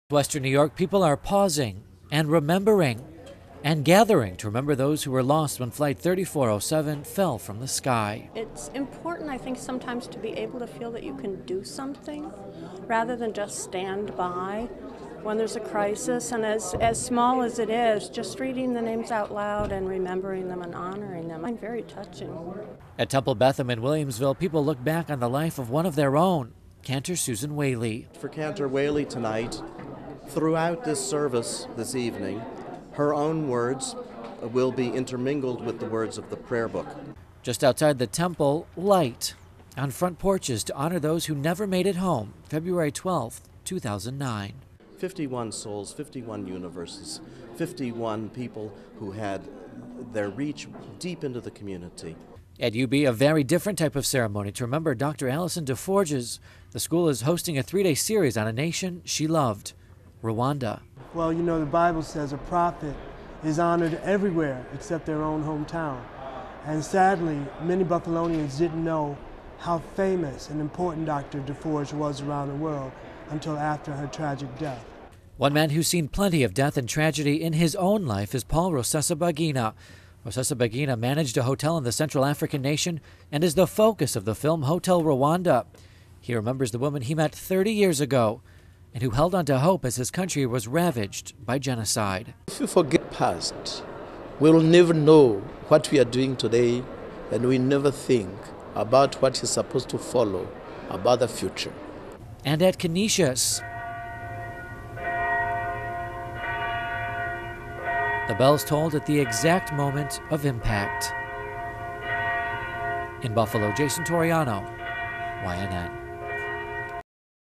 WEB 3407 Cermonies.mov
he walk was just one way people across the community took time Friday to honor what happened in Clarence Center one year ago --- a snowy February night where 50 people lost their lives.YNN's Jason Torreano tells us how one year later, in temples, at universities and on porches - people are remembering flight 3407.

*The story was written, edited, reported and shot by YNN anchor/video journalist Jason Torreano.